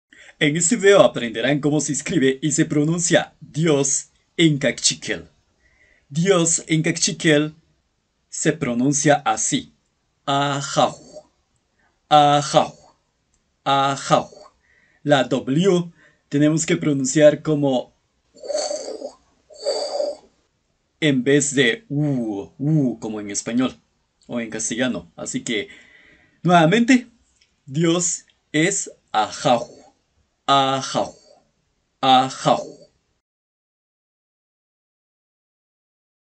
Como se dice DIOS en KAQCHIKEL
¿Cómo se dice DIOS en kaqchikel? AJAW También en este vídeo escucharán como se pronuncia y ver la explicación, así que vean el vídeo completo.